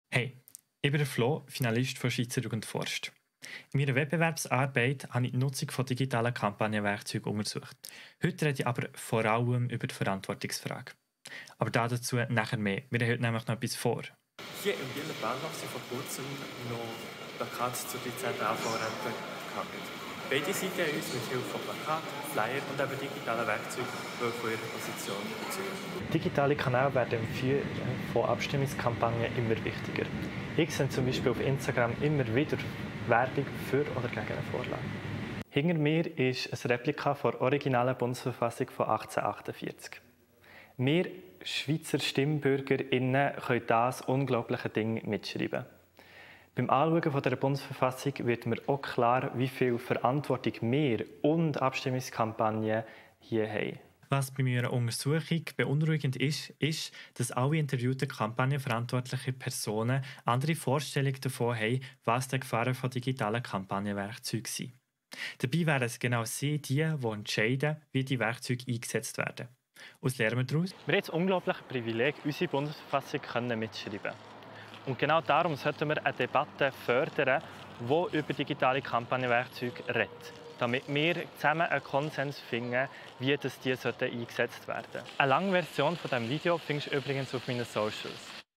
Parteipolitische Unterschiede beim Einsatz digitaler Kampagnenwerkzeuge in Abstimmungskampagnen
Florian Hebeisen NWB24